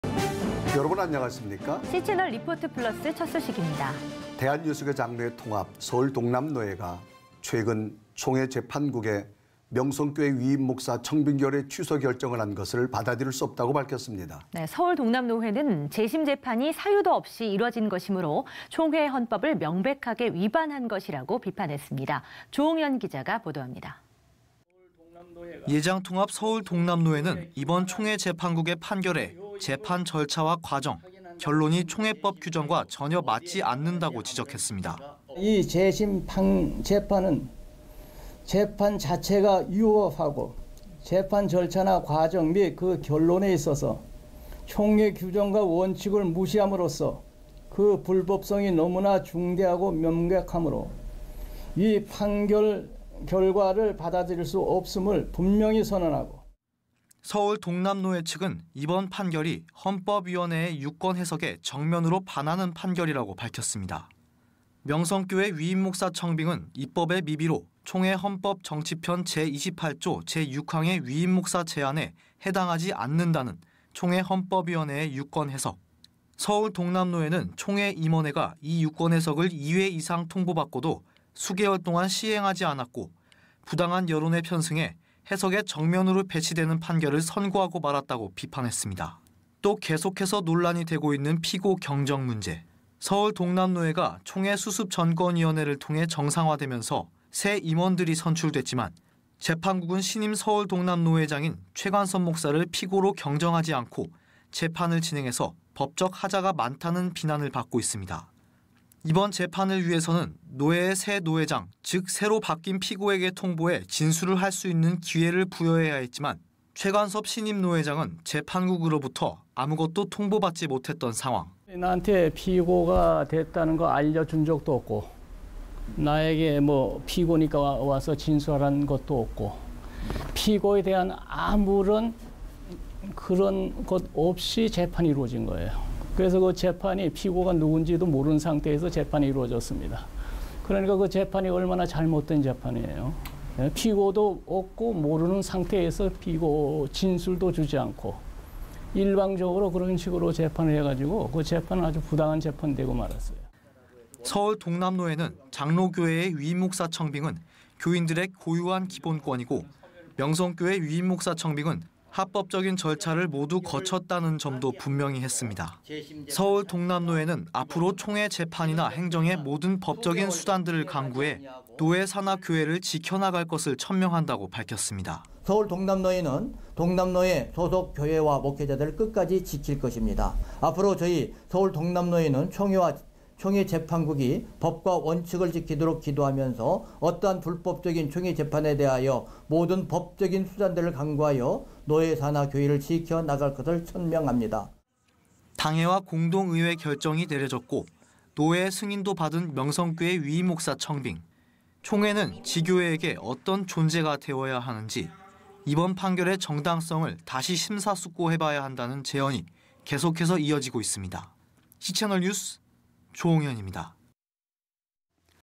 서울동남노회, 재심재판, 불법성 중대하고 명백해
서울동남노회가 대한예수교장로회 통합 총회 재판국의판결에 대해 입장을 발표했습니다. 명성교회 위임목사 청빙 결의 취소 결정을 받아들일 수 없으며, 이는 총회헌법을 명백하게 위반 한 것이라고 비판했습니다.